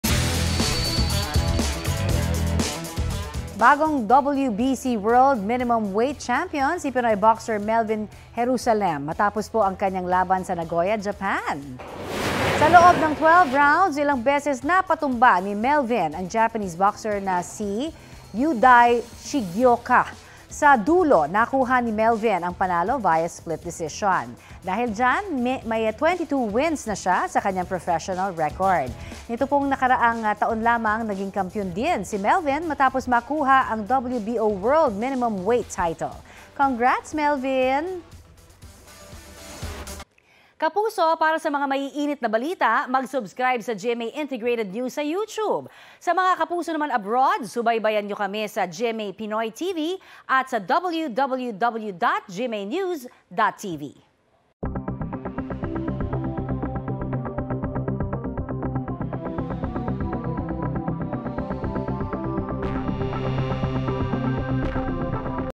Pinoy boxer Melvin Jerusalem, nakuha ang WBC World Minimumweight Championship sa Nagoya, Japan | BT
Bagong WBC World Minimumweight Championship si Pinoy boxer Melvin Jerusalem!